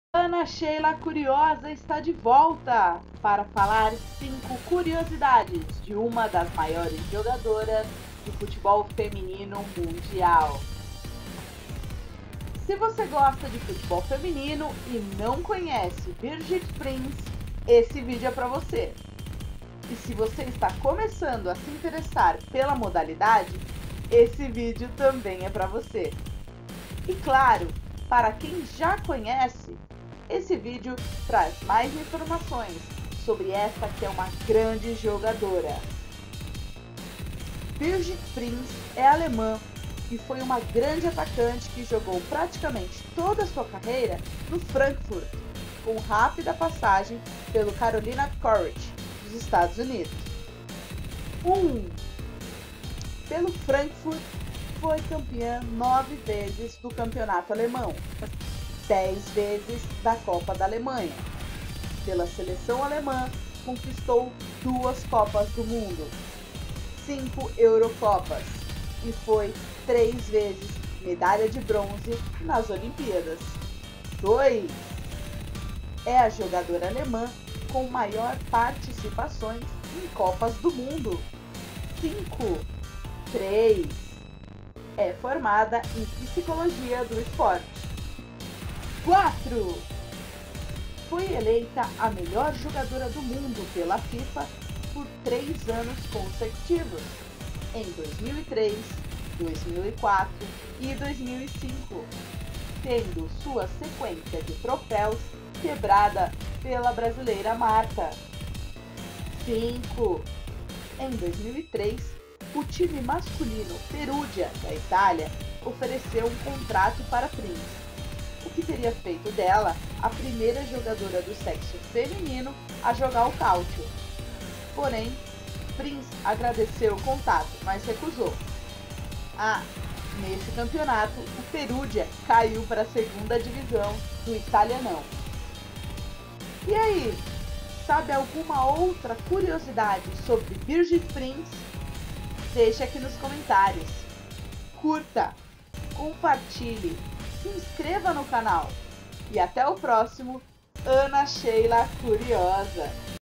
5 CURIOSIDADES SOBRE BIRGIT PRINZ - Quase jogou o Calccio
A bola da vez, Birgit Prinz, ex-jogadora alemã que rivalizou com Marta durante sua carreira. Artilheira nata.